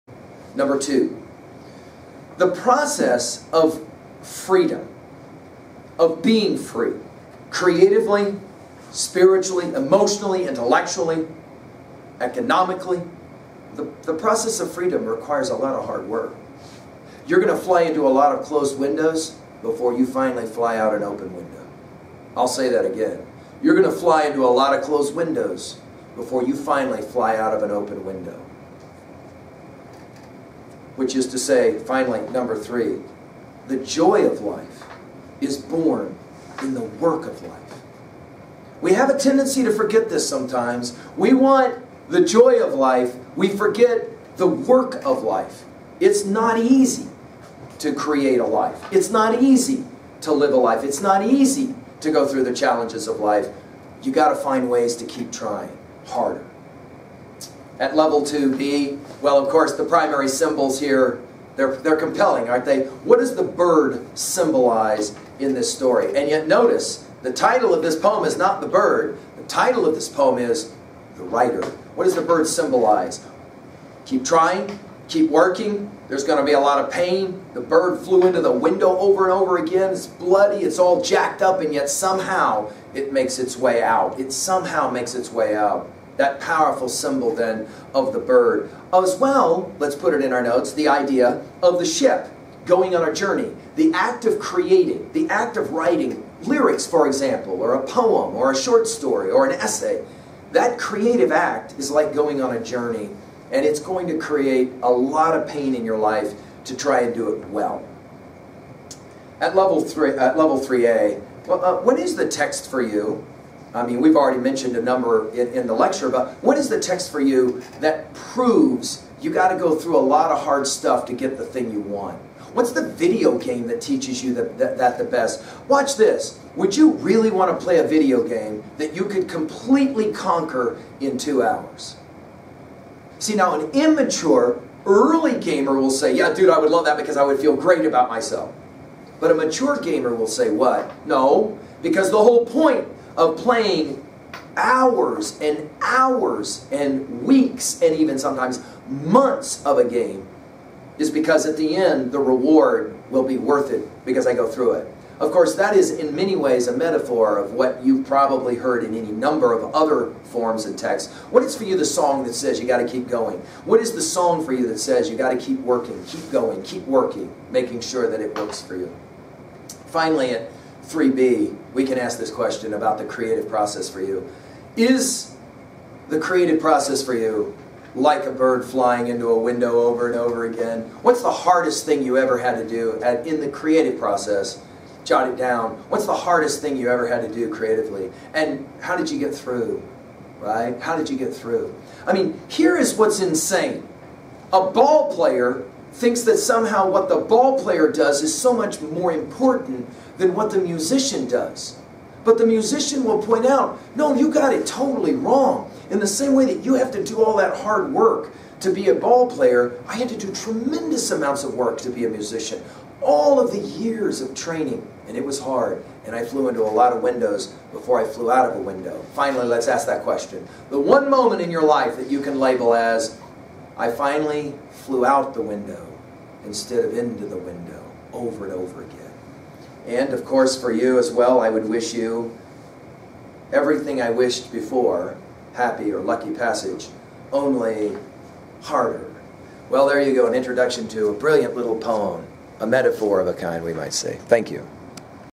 Richard Wilbur’s The Writer, B
McGee of 303 and Learnstrong.net lectures on Richard Wilbur’s The Writer, B